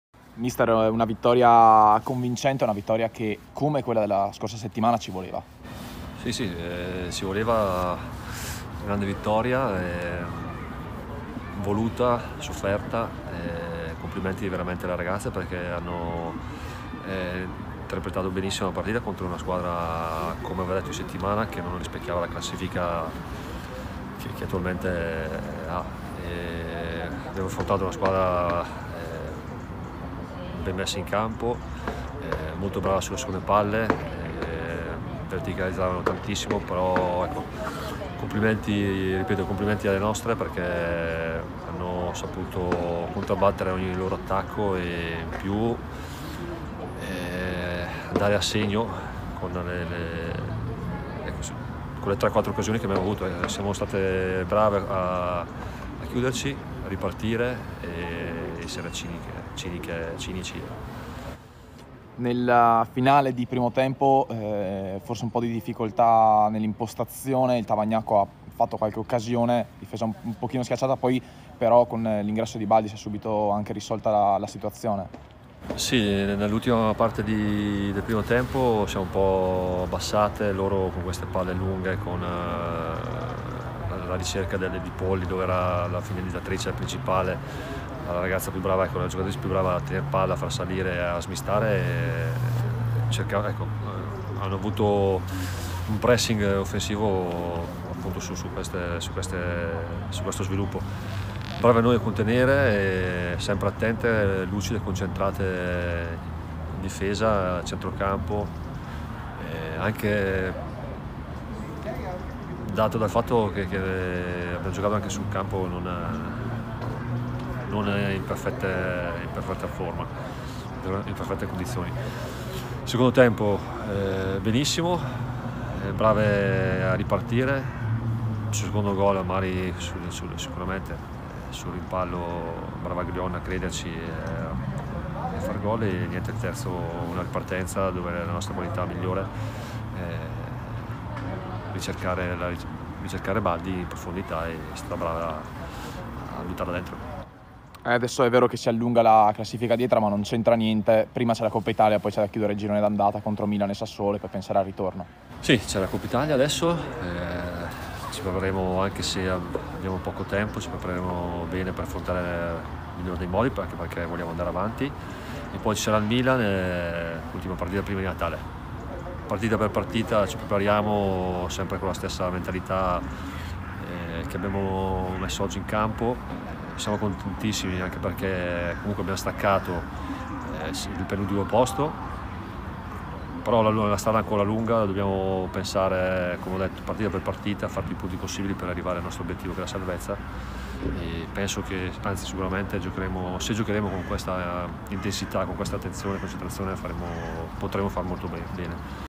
HVW / Bonazzoli: «Grande vittoria, complimenti alle ragazze! Ora pensiamo a San Marino e Milan»
Le principali dichiarazioni dell'allenatore gialloblù Emiliano Bonazzoli, rilasciate al termine di Tavagnacco-Hellas Verona, 9a giornata della Serie A TIMvision 2019/20.